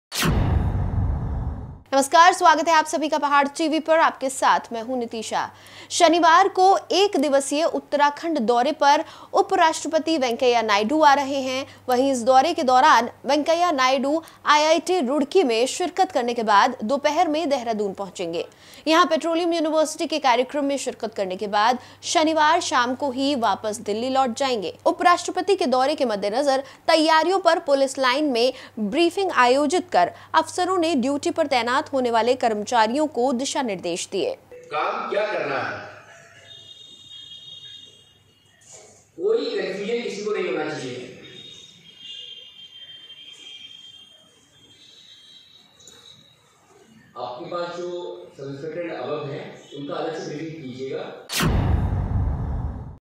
शनिवार को उत्तराखंड के दौरे पर रहेंगे उपराष्ट्रपति वेंकैया नायडू | Pahad TV
शनिवार को उत्तराखंड के दौरे पर रहेंगे उपराष्ट्रपति वेंकैया नायडू